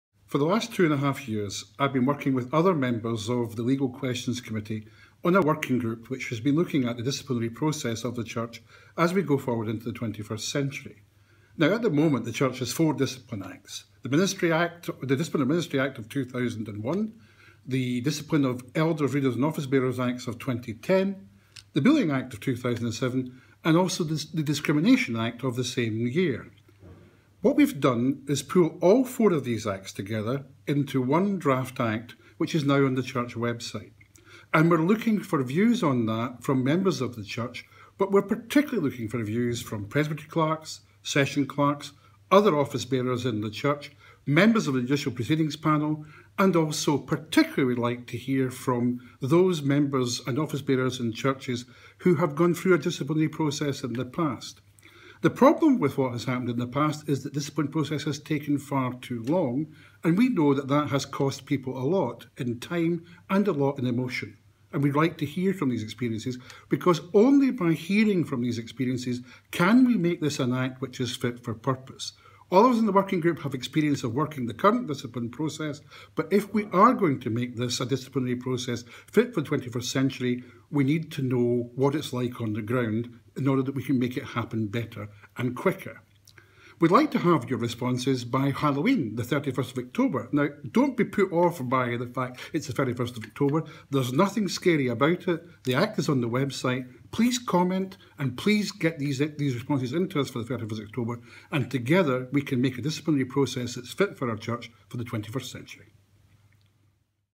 Comments sought on new Church Disciplinary Act
Robert Hynd explains why the Church is bringing in a new Disciplinary Act, and why the Legal Questions Committee is seeking comments on the draft Act and how it might be applied if it is brought into force next year. Comments must be submitted by October 31st. He says “We are particularly keen to hear from Presbytery Clerks, Members of the Judicial Proceedings Panel, Parish Ministers, former Respondents, Elders, and importantly, anyone affected by an earlier disciplinary process. ”